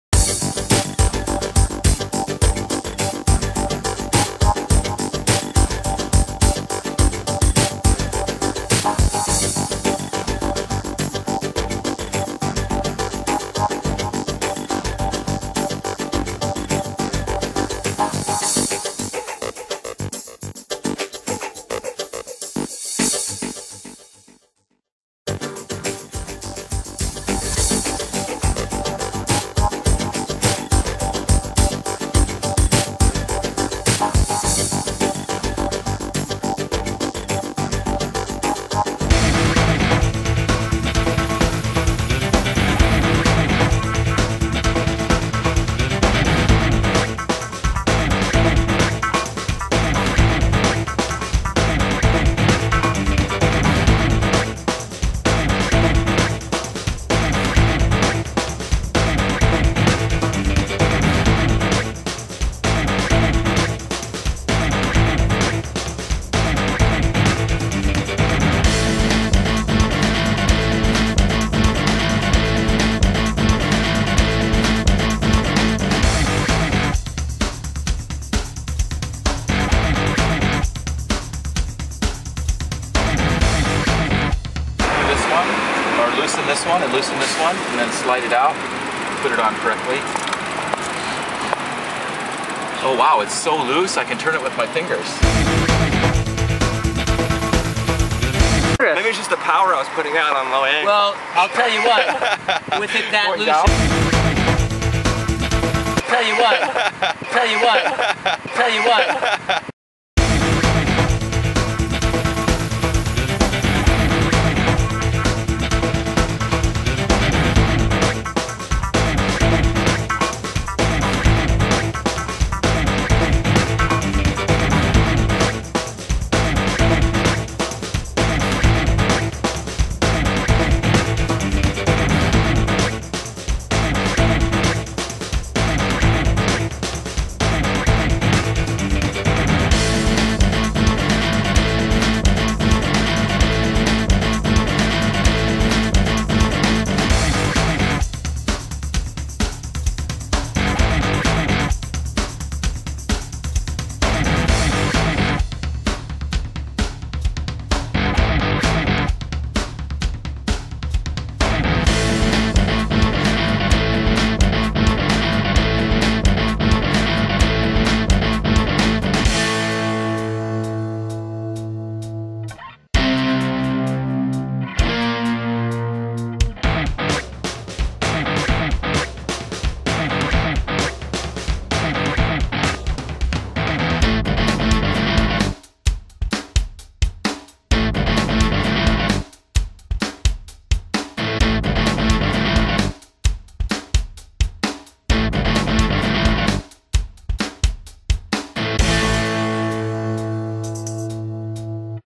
CTS v1
This video will provide some useful tips for cyclists.  These tips are designed to help you when  you are not on the bike, and to promote more cycling or prevent you from quitting all together.